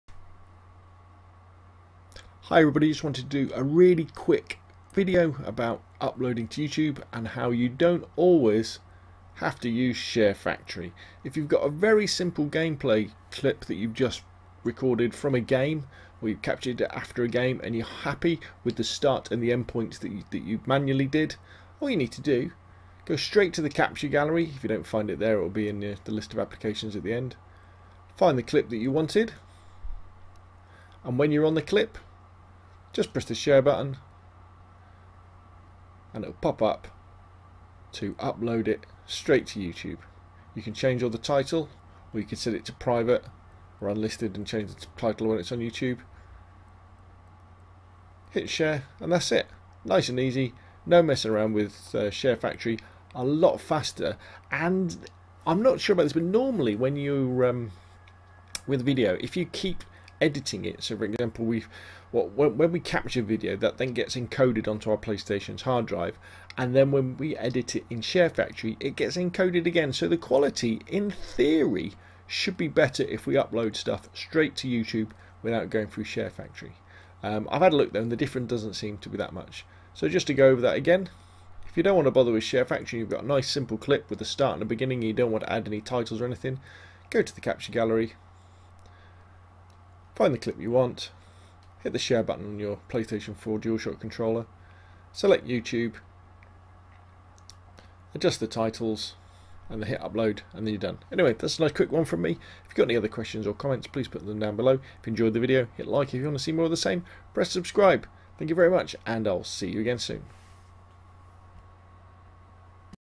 Upload Game-Play Videos To YouTube From PS4 Without Sharefactory
A quick tip to save time when uploading simple game-play videos where you're happy with the start and end points, and don't need to edit the footage.
Thanks, Rob.